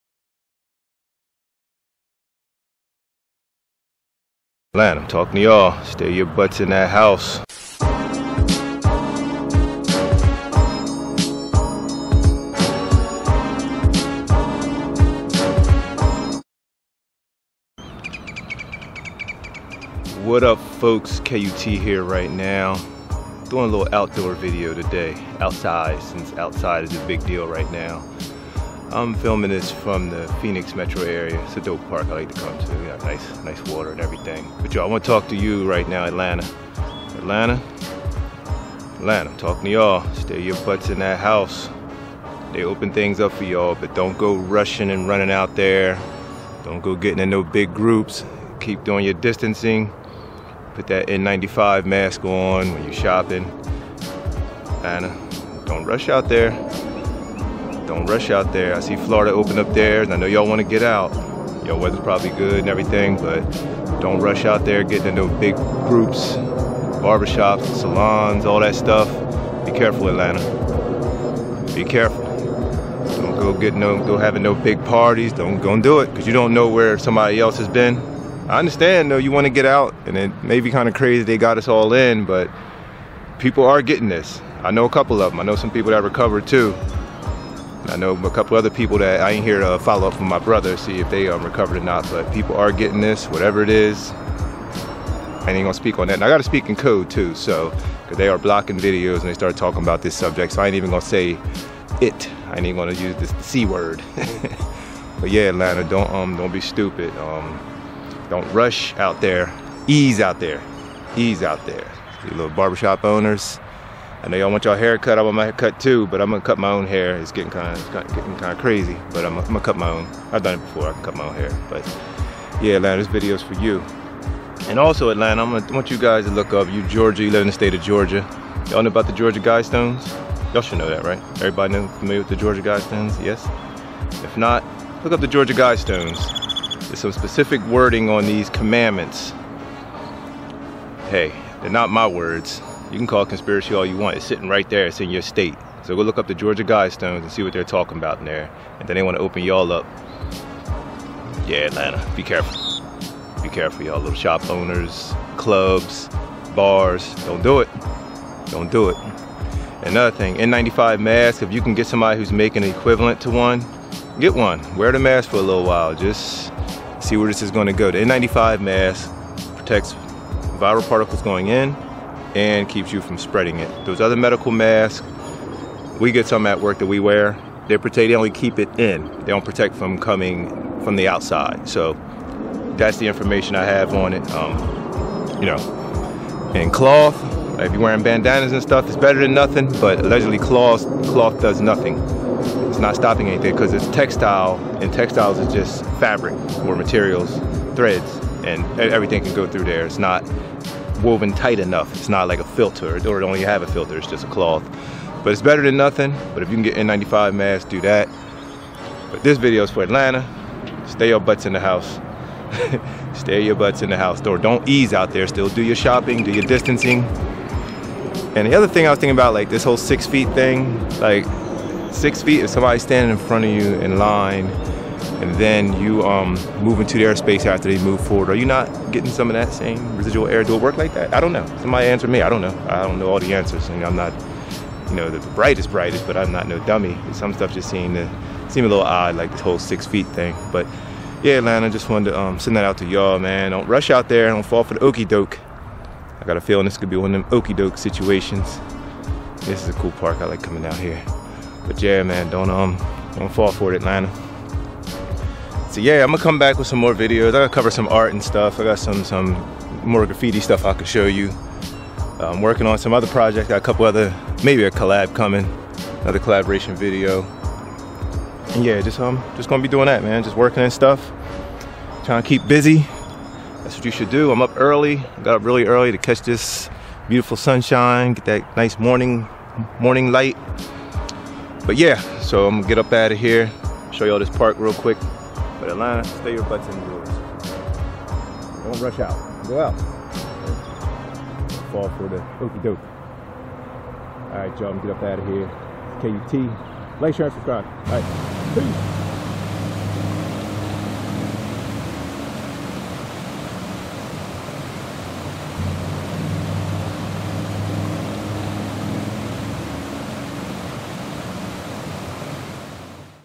Georgia Reopens! Be Careful Atlanta, ATL Folk Listen UP!
This is a warning to the state of Georgia residents specifically Atlanta. I'm telling the residents to be slow to come out. Do immediately get in a hurry to be around each other. Deaths are still occurring. The residents should er on the side of cause until things level off. Continue to wear the mask and gloves while still practicing social distancing.

The Stolen Art Show Documentary

We will bring you Hip Hop, Art, News, Vinyl Records, Interviews, Reviews, Event Coverage and The Greatness in All Cultures.